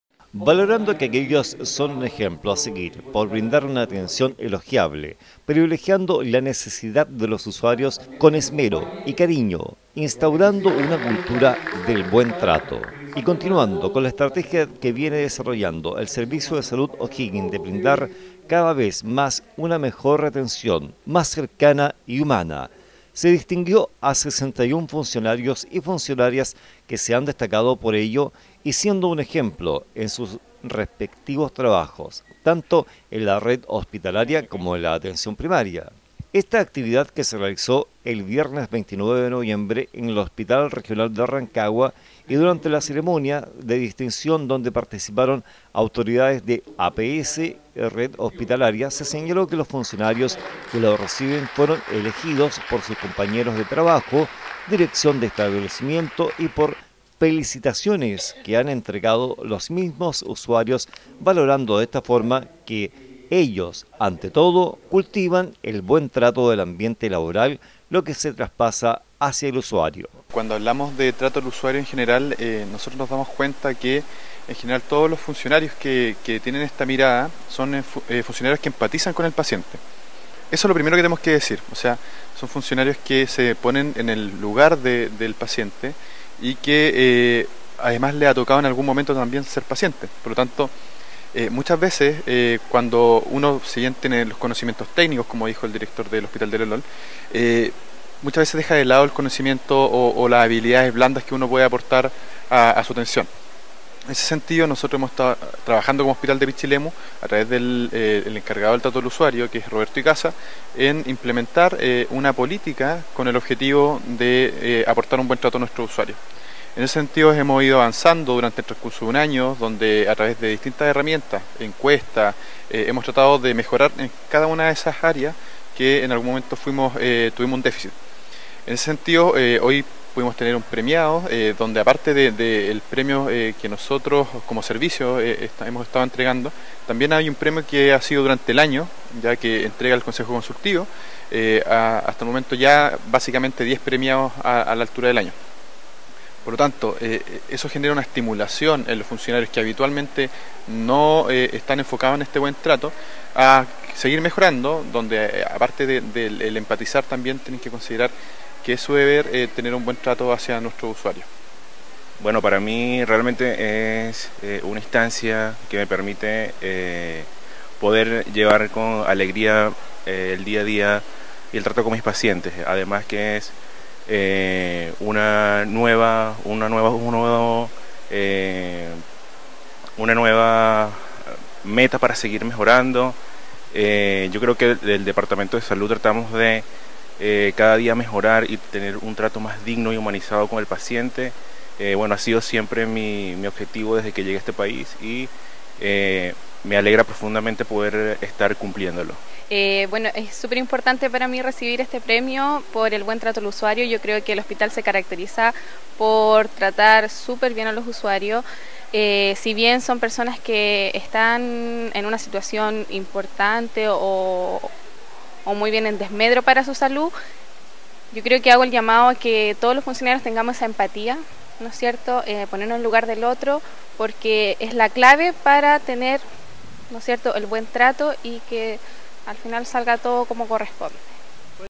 Servicio de Salud O’Higgins destaca a 61 profesionales por el Buen Trato
Valorando que ellos son  un ejemplo a seguir por brindar una atención elogiable,  privilegiando la necesidad de los usuarios con esmero y cariño, instaurando una cultura de buen trato. Continuando con la estrategia que viene desarrollando el Servicio de Salud O’Higgins de brindar cada vez más una mejor atención, más cercana y humana, se distinguió a 61 funcionarios y funcionarias que se han destacado por ello, siendo un ejemplo en sus respectivos trabajos, tanto en la red hospitalaria como en la atención primaria.